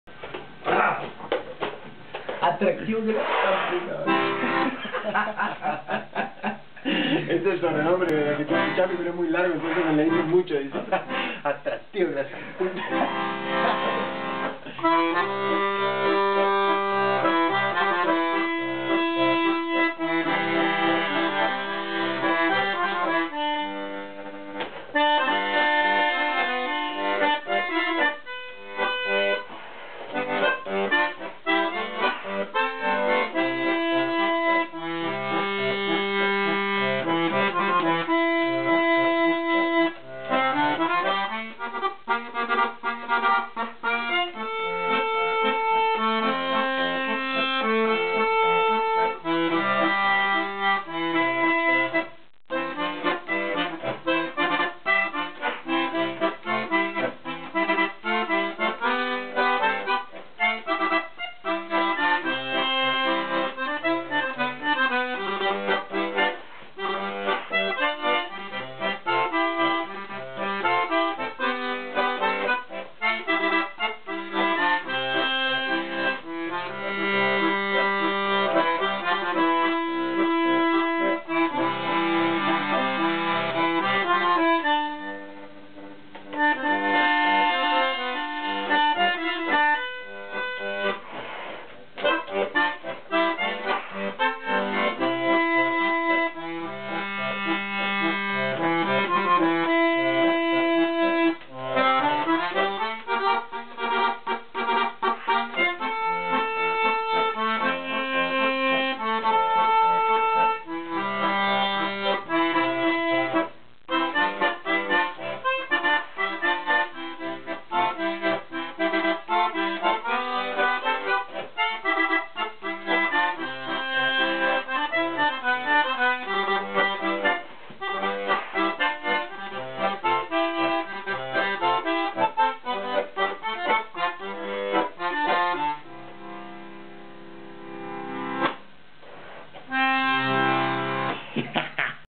tamandua rugby y chamame!!!
chapi unas de las mejores incorporaciones de tamandua rugby en el 2008 nos muestra otra de sus virtudes innatas de un buen entrerriano!